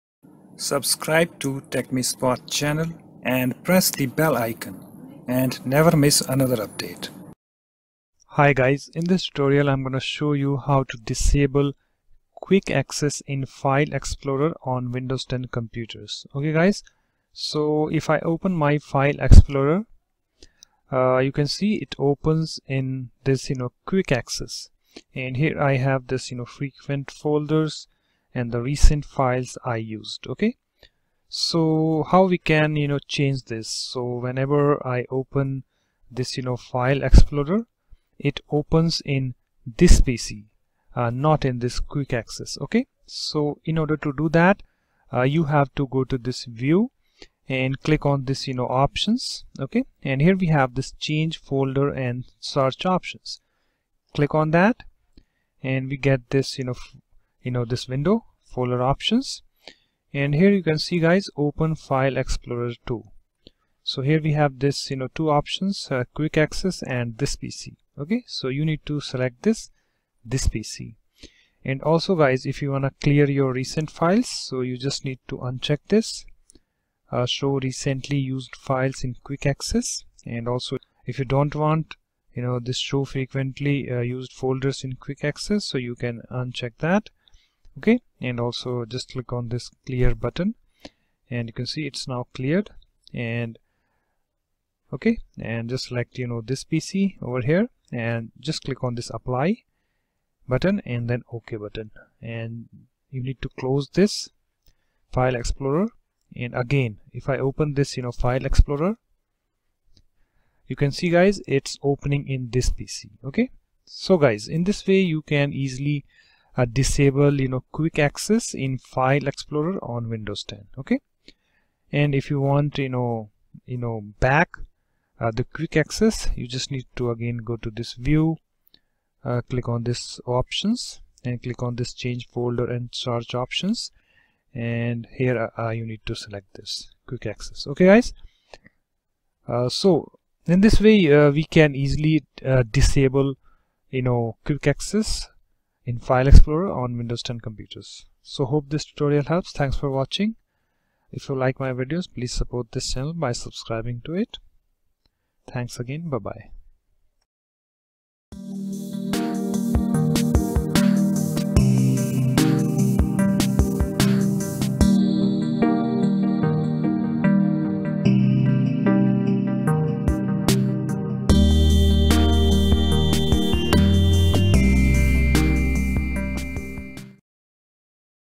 Windows 10 Tutorial: How to Disable Quick Access in File Explorer
Step 1: Open File Explorer.

Step 2: Click on 'View' in Menu bar.

Step 3: Click on 'Options'.

Step 4: Click on 'Change folder and search options.'

Step 5: Click on the drop down 'open file explorer to.'

Step 6: Click on 'This PC.'

Step 7: Click on 'Apply' and 'Ok' button.

Close the file explorer and when you open it again, it opens in 'This PC.'

It's Done